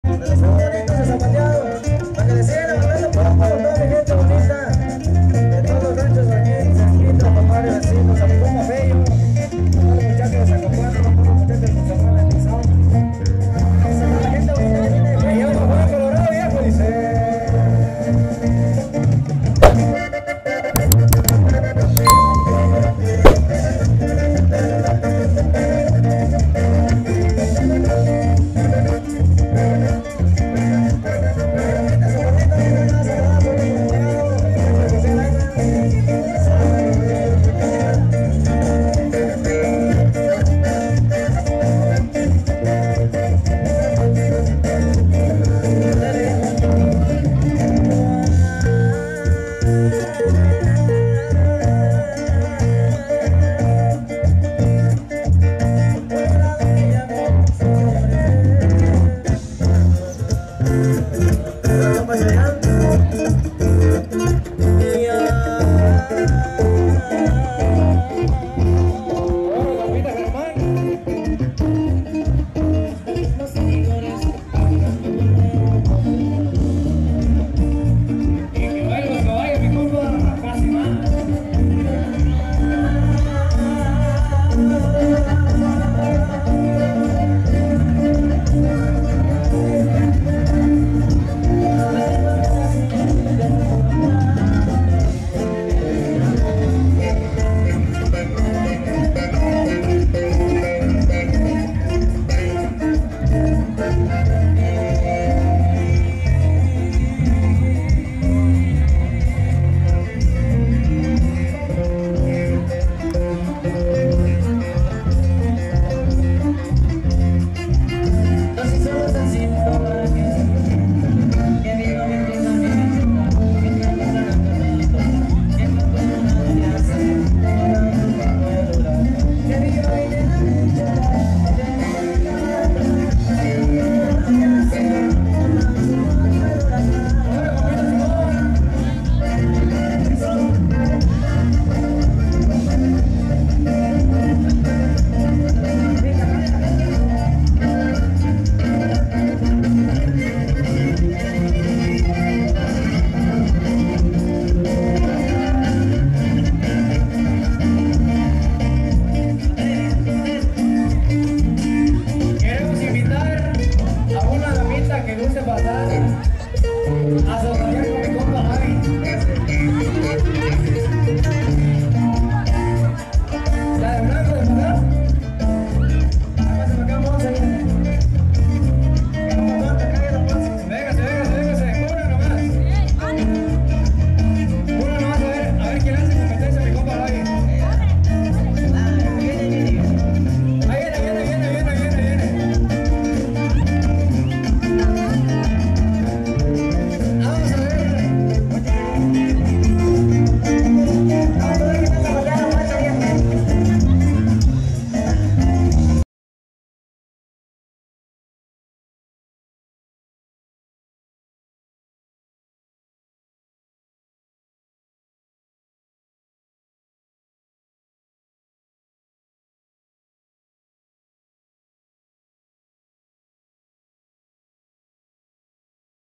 La CHICA del vestidito ROSA toda una MARAVILLA un ENCANTO en este bello ZAPATEADO 😍
Amigos aqui les dejamos este bonito zapateado en Cutzamala, Guerrero. 💃🎀🔥😍📹